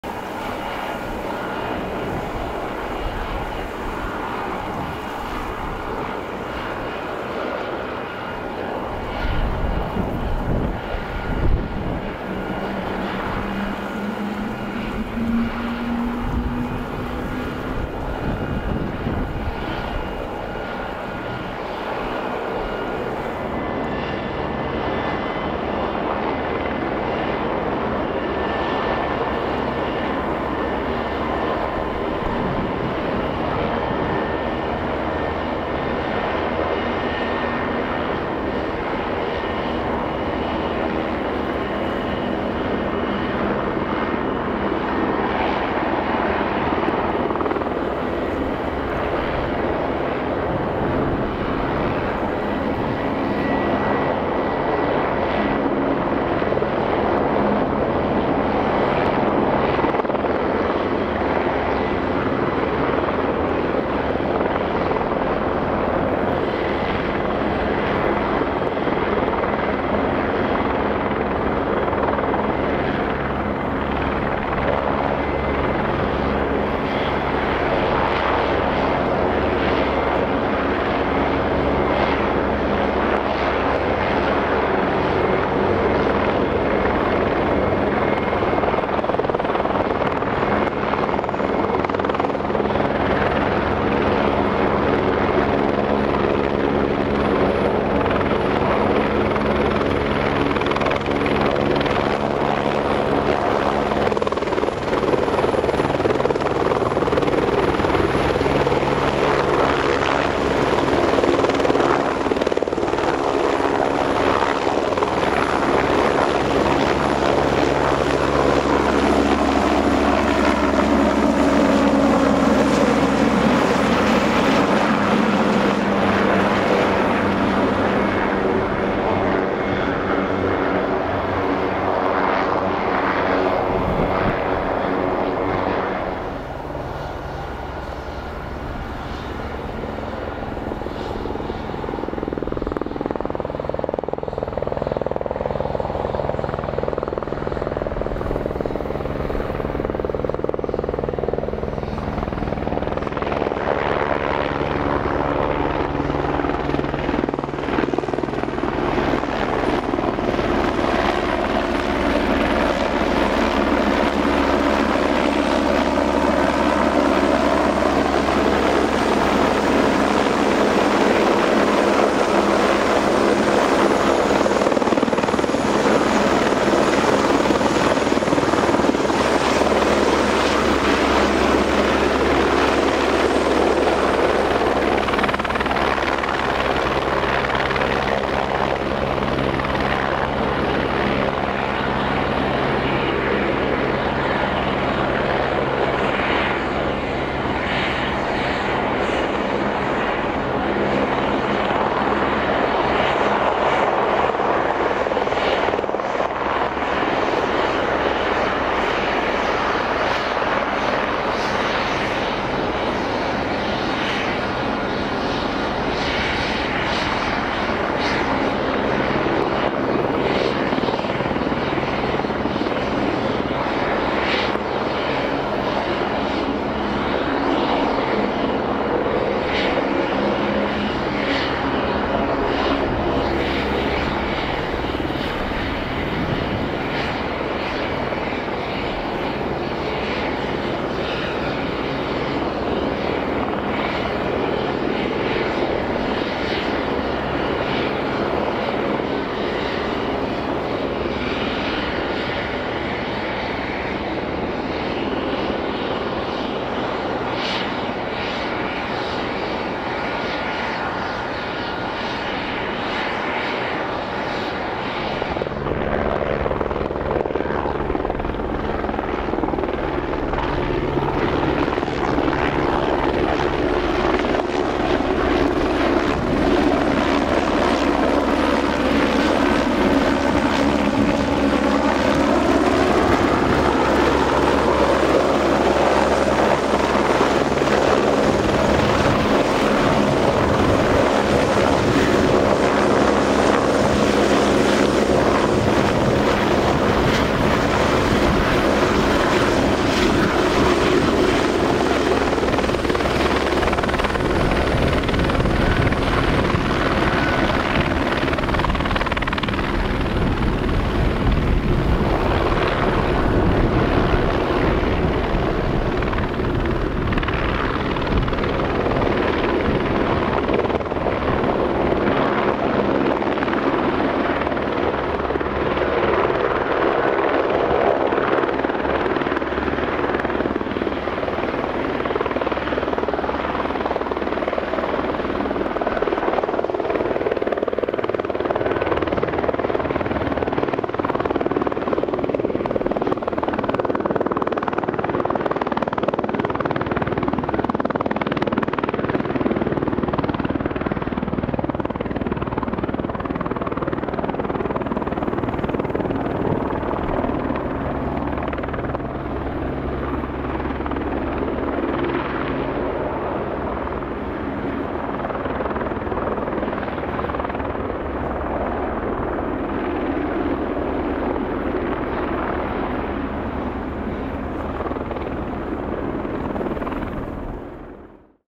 【ヘリコプター】 ６軸スタビライザー SHOTOVER K1,F1 を搭載時のヘリコプター HD 1080p
SHOTOVER K1を搭載したユーロコプター AS350 JA6508と SHOTOVER F1を搭載したアエロスパシアル JA9731の離着陸シーン 東京ヘリポートにて。